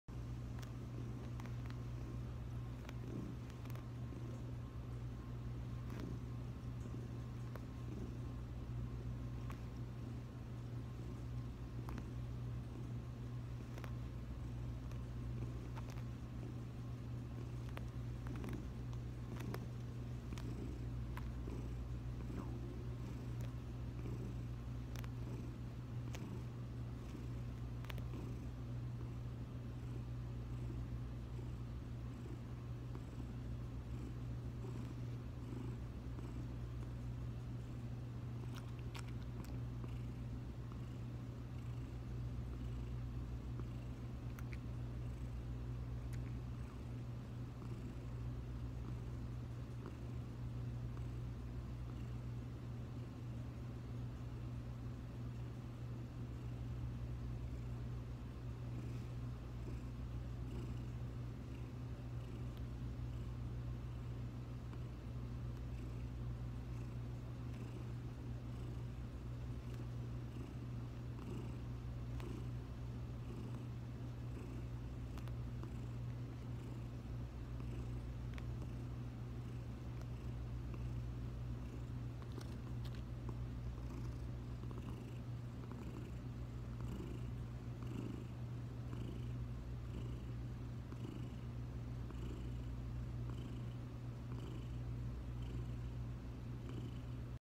The First Time Nayna Saw Herself
You know when you’re in that zen mode of mushing your human’s fat and you start drooling all of the sudden you see your own reflection in the phone and... totally buzz kill!